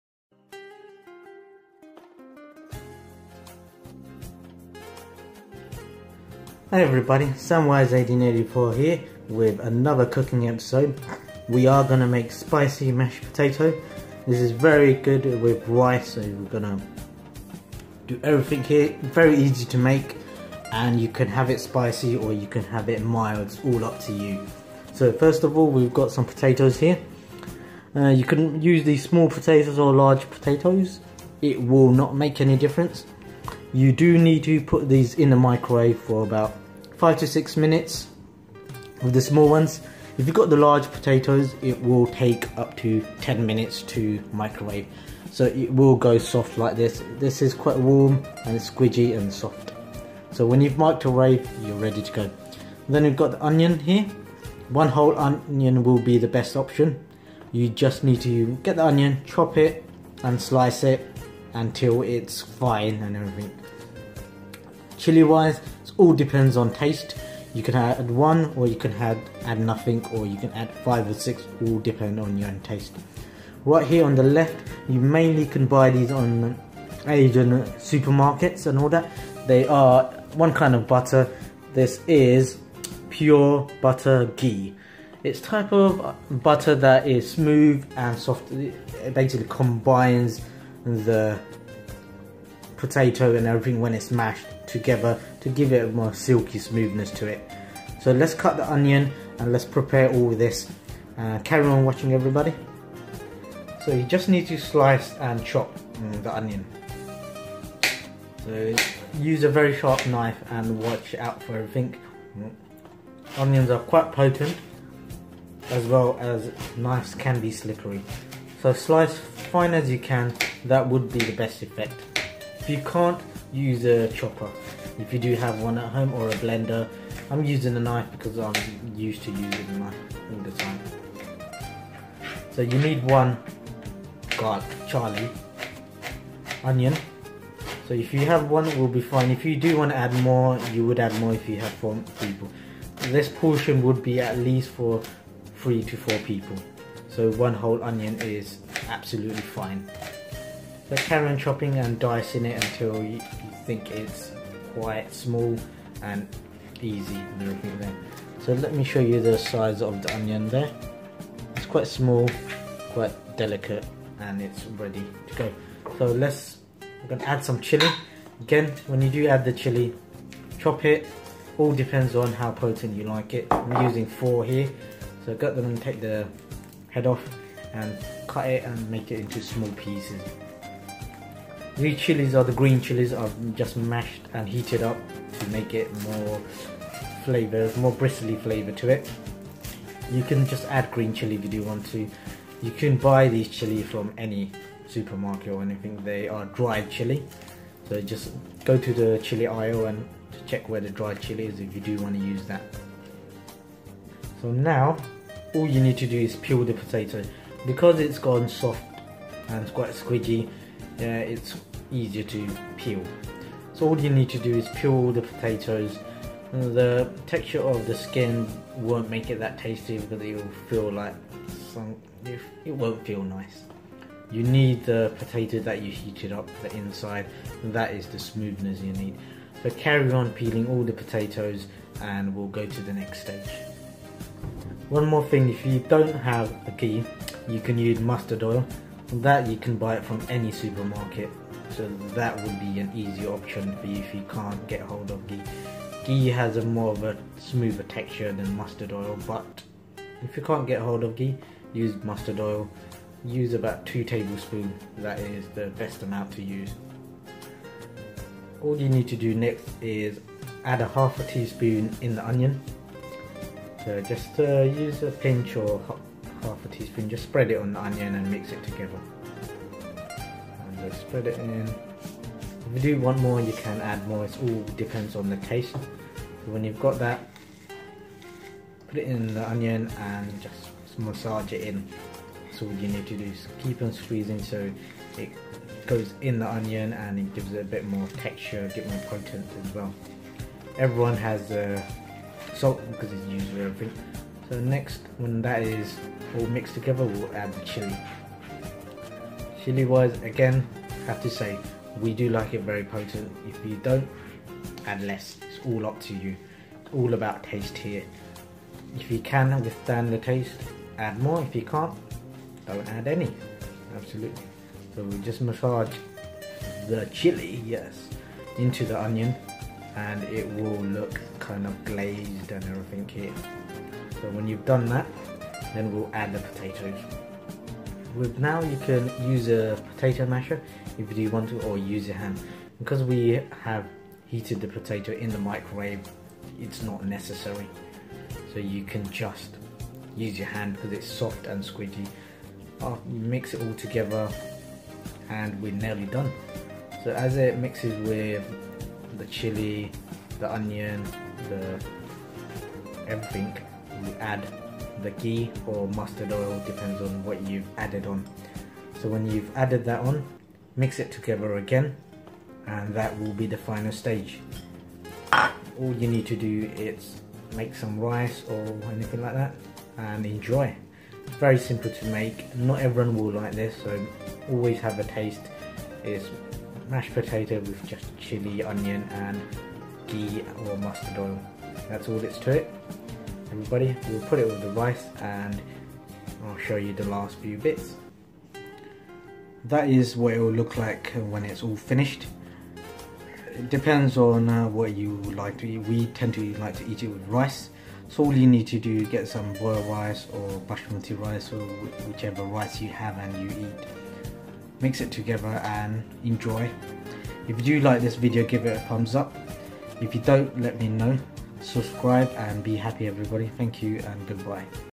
Spicy mash potatoes
Indian style spicy mashed potatoes for dinner with rice

Dozen small or medium potatoes
1 medium size onion
dried chilli depending taste
Mustard oil or ghee
5 to 6 fresh coriander leaves